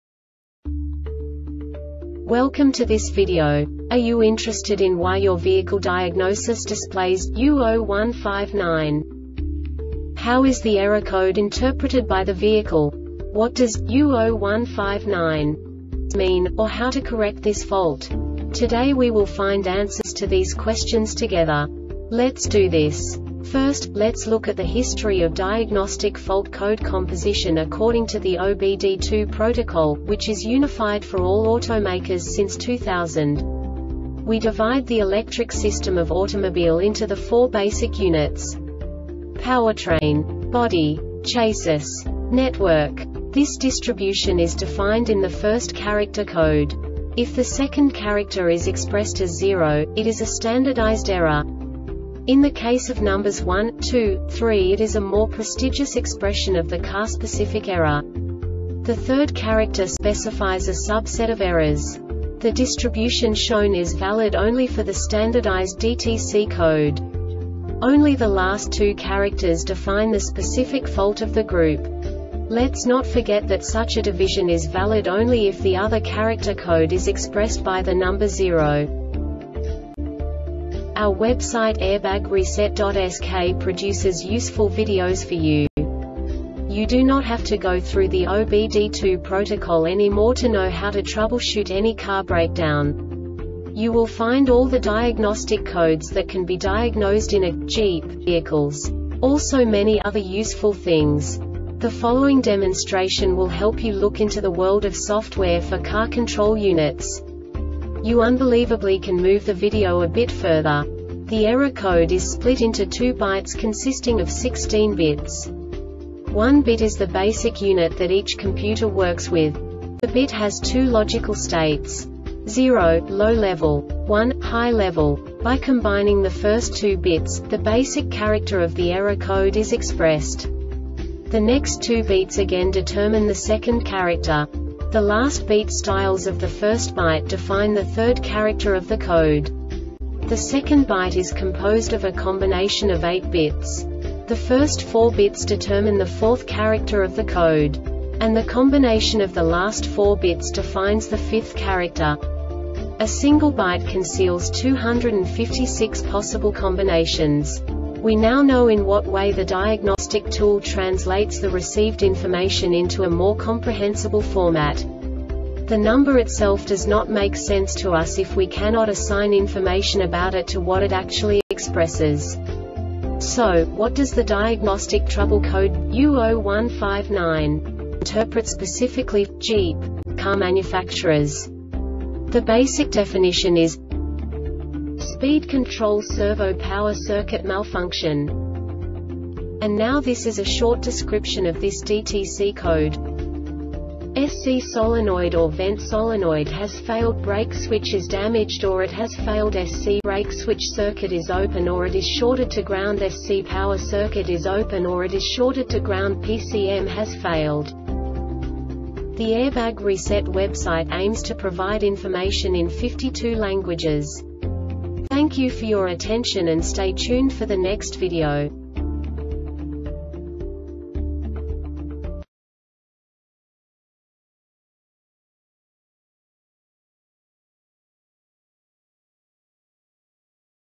DTC Jeep U0159 Short Explanation
Contents:
0:21 Basic DTC analysis according to OBD2 protocol standard.
1:48 Insight into programming
2:58 A diagnostically understandable description of the Diagnostic Trouble Code
3:05 Basic error definition

Make:
Jeep
Code:
U0159
Definition:
Speed Control Servo Power Circuit Malfunction
Cause:
S or C solenoid or vent solenoid has failedBrake switch is damaged or it has failedS or C brake switch circuit is open or it is shorted to groundS or C power circuit is open or it is shorted to groundPCM has failed